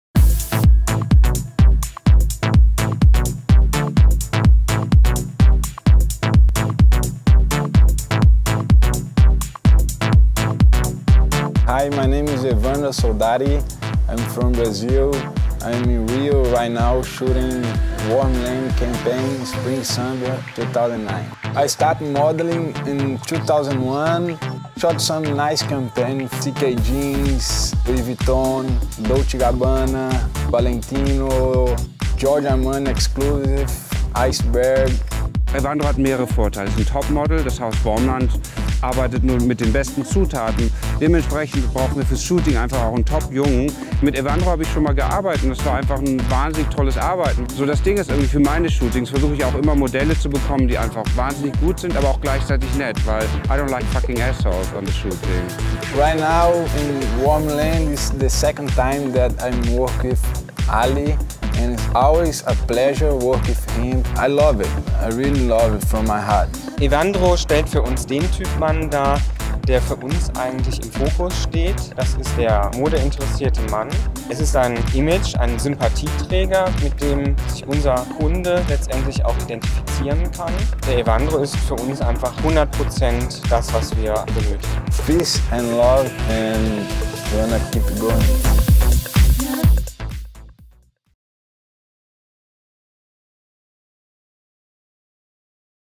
Wormland S/S 2009_ Behind-the-Scenes_Evandro Soldati
Evandro Soldati behind the scenes of Wormland Metamorphose photo shoot by Ali Kepenek in Rio de Janeiro January 2009. Creative Direction Dirk Koeberle and Art Director Michael Meise Interview. Styling Tobias Frericks, Make-up/Hair Regina Harris, Camera and Video Edit Nico Vogelsang, Thomas Kiel